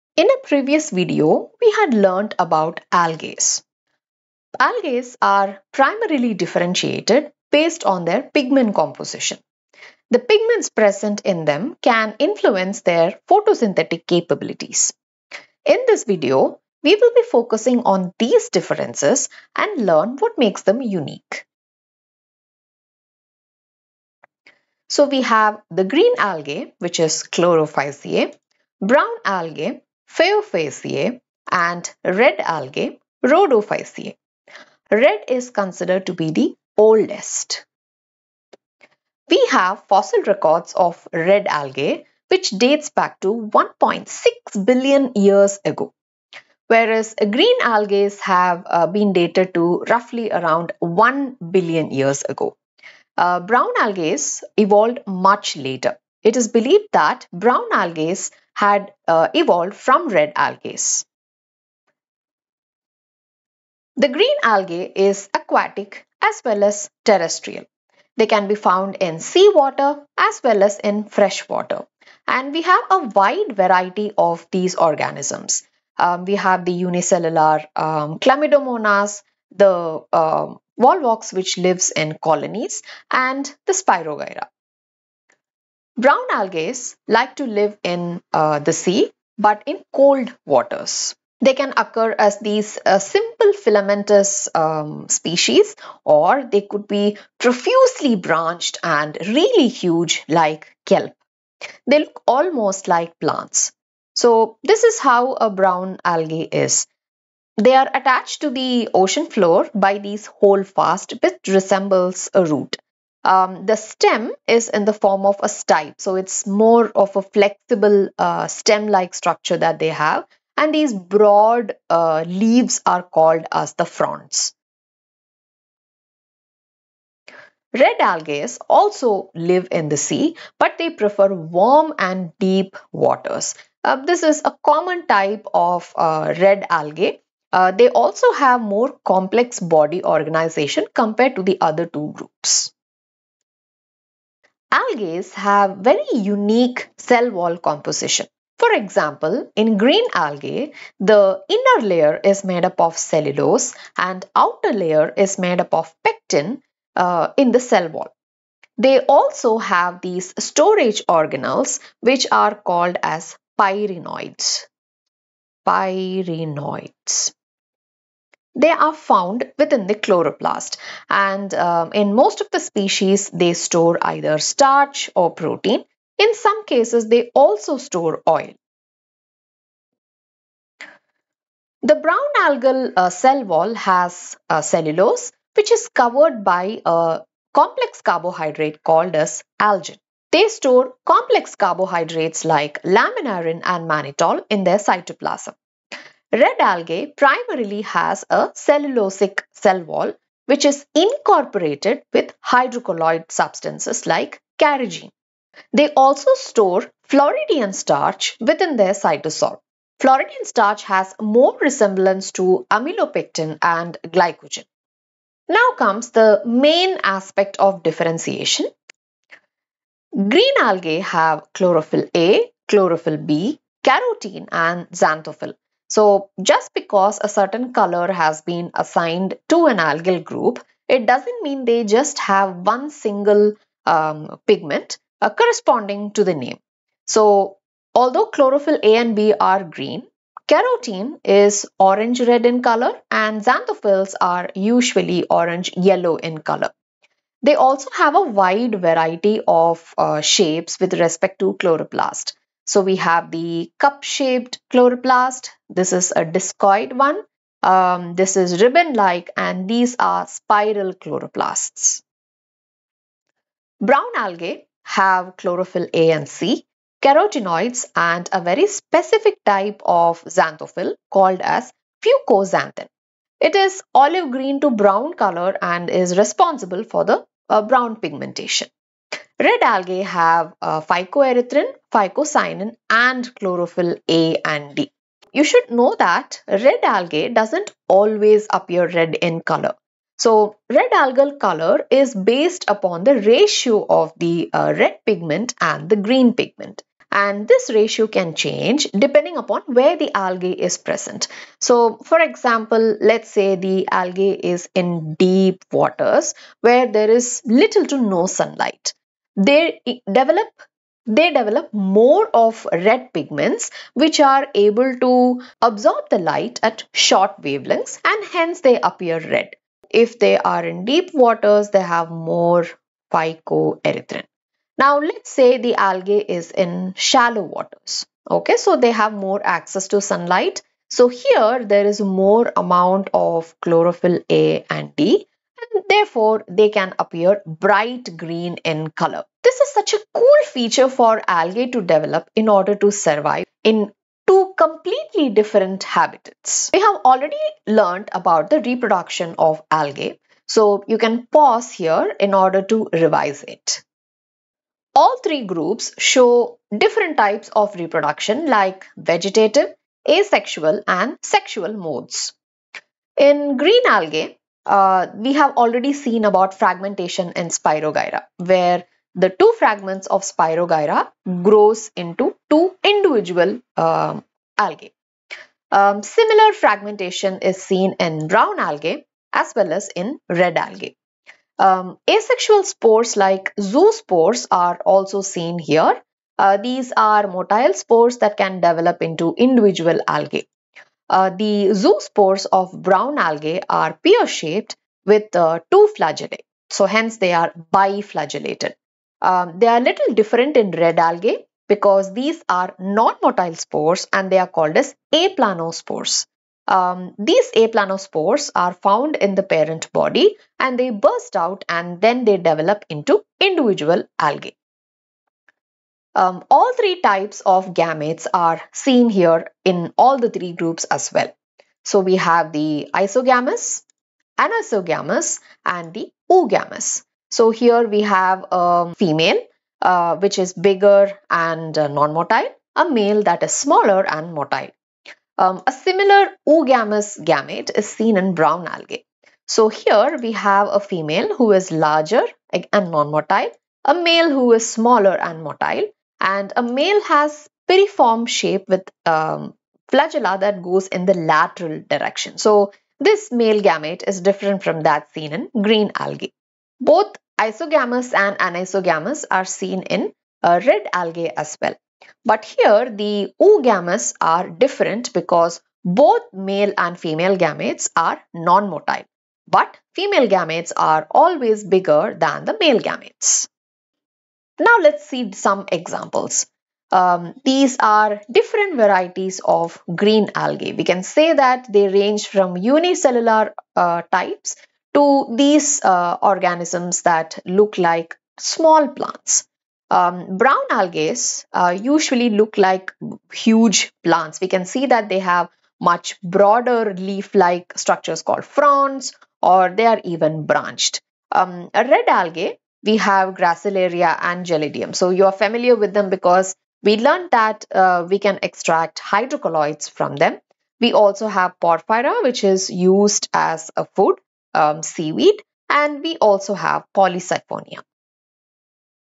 Classification of Algae | Plant Kingdom | Grade 11 | Biology | Khan Academy
Green, brown, red - algae come in more varieties than you might think. The video breaks down how these ancient organisms are classified beyond just their colors. We explore the differences in their habitats, cell wall composition, and storage methods that set each group apart. Learn about the distinct photosynthetic pigments that define major algal groups and compare their varied reproductive strategies. From simple pond scum to complex seaweeds, see examples of each classification and understand what makes them unique in the biological world.

00:00 - Introduction
00:24 - The three groups
01:03 - Habitat, algal varieties
02:24 - Cell wall composition, Storage products
03:35 - Photosynthetic pigments
05:47 - Reproduction in algae - recall
05:55 - Comparison of reproduction between algal groups
07:55 - Examples for each group

Courses on Khan Academy are always 100% free. Start practicing—and saving your progress—now!

Created by